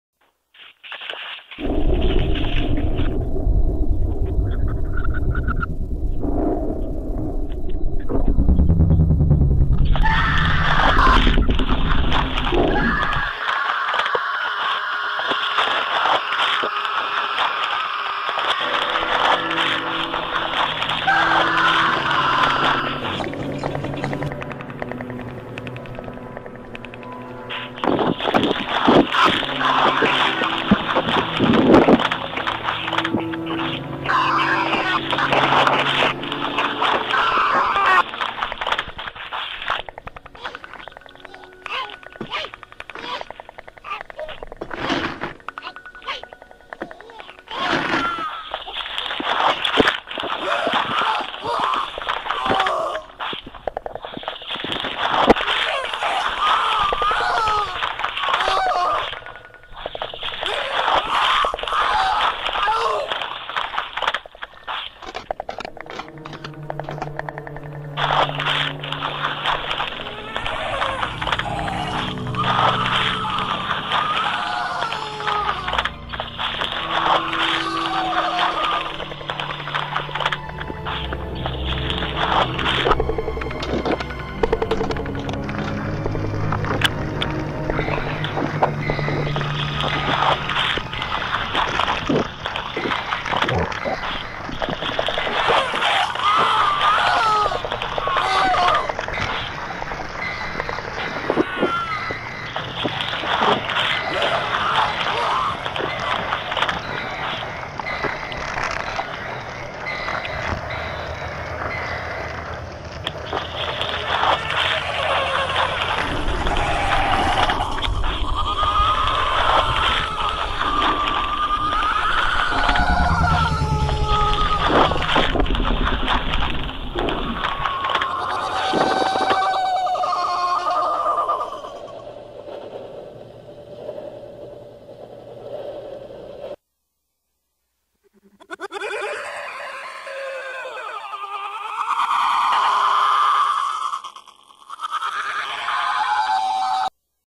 Bigfoot is a Drag
Footage of an attack ... dragged by a Bigfoot